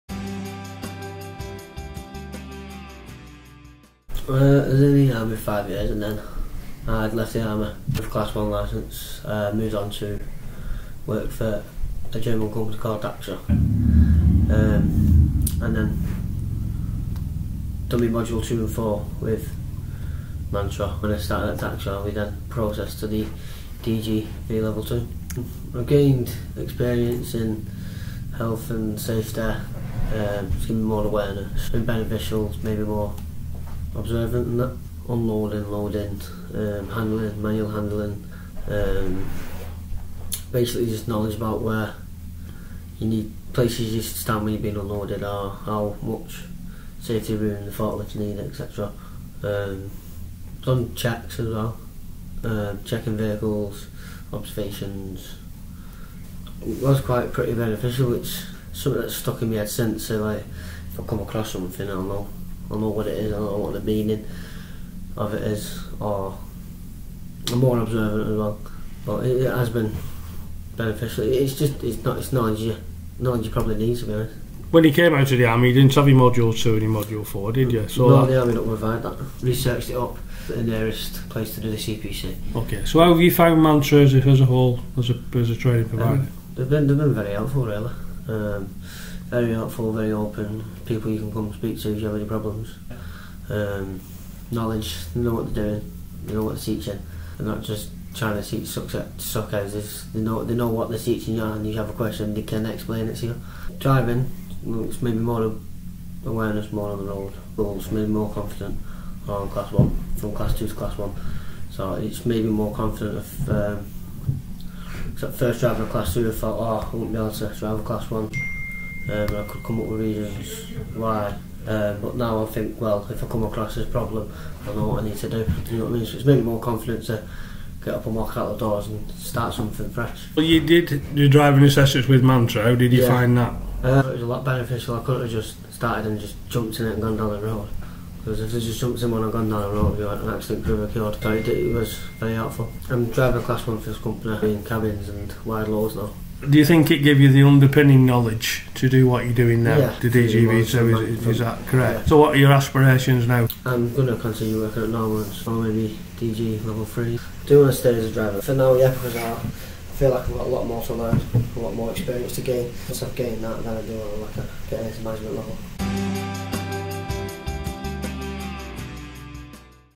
John O'Connor, Class 1 DGV Apprentice
John left the Army and wanted to upgrade his skills to become a Class 1 DGV Driver.